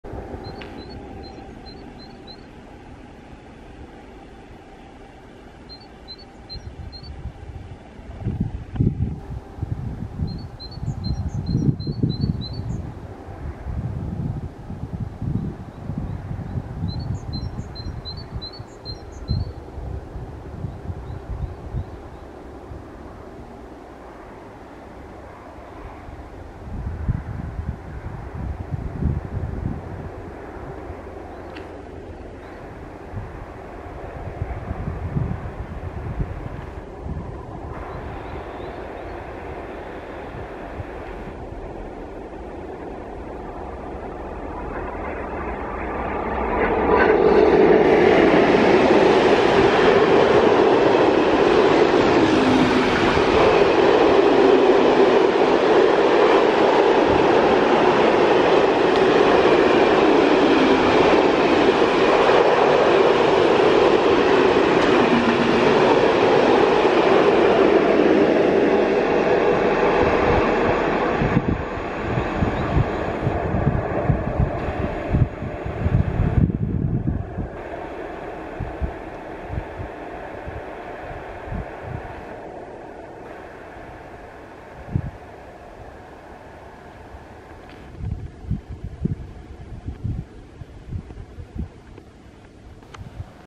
Lokomotiva 363 043-1 a 363 022-5 - 1.4.2022
Nákladní vlak vedený lokomotivou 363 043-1 a tlačený postrkovou lokomotivou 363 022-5 projíždí zastávkou Žichlínek ve směru Zábřeh. Natočeno v pátek 1.4.2022 v 11.38 hodin.